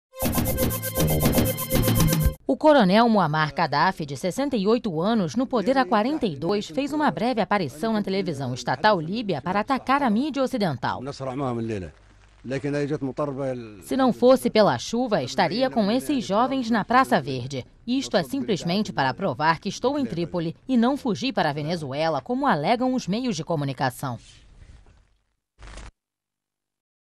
Kadhafi reaparece
O ditador líbio Muamar Kadhafi mostrou-se decidido a permanecer no poder, horas depois de suas forças de segurança terem protagonizado os mais violentos momentos da repressão contra os protestos que exigem sua renúncia, após 42 anos no poder. Segundo o canal de televisão oficial do país, Kadhafi vai falar ao povo em um discurso na televisão em breve.